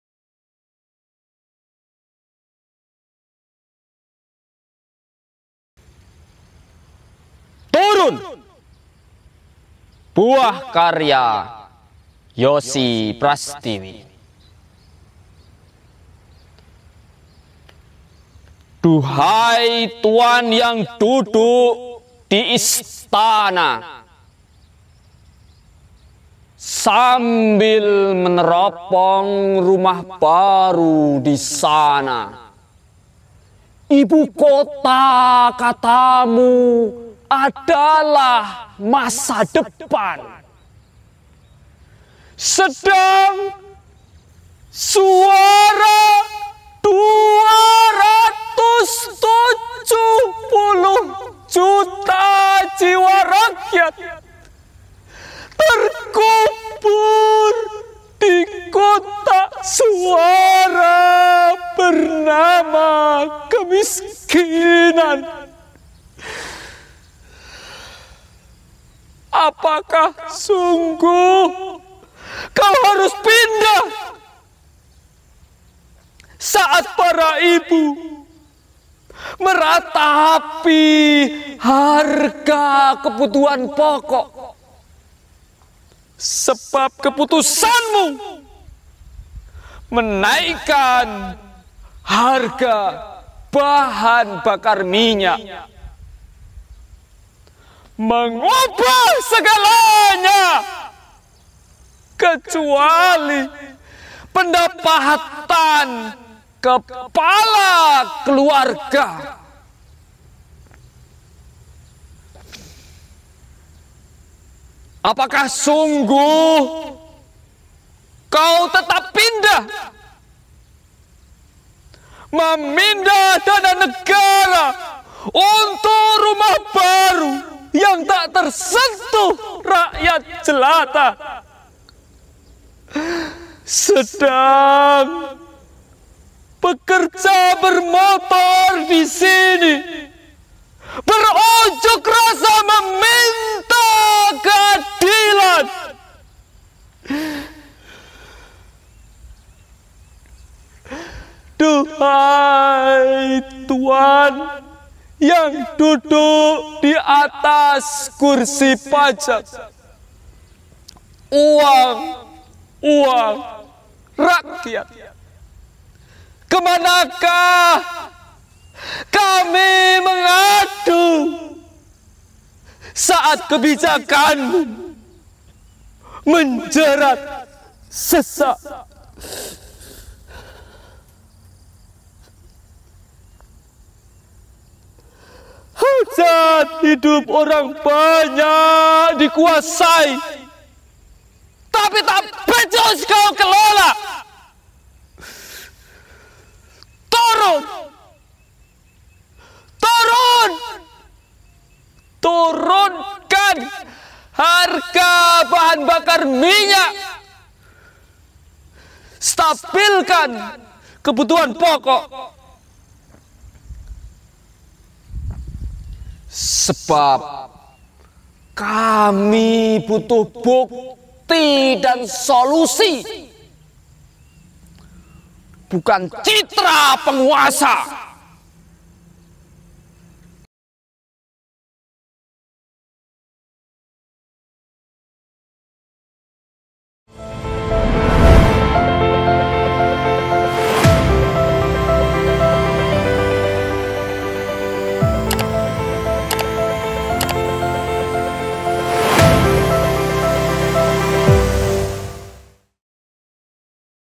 Anak Muda Jogja Demo BBM Naik lewat Puisi "Turun"
Puisi "Turun" karya Yosi Prastiwi ini merupakan ungkapan jeritan masyarakat atas kebijakan pemerintah menaikkan harga BBM.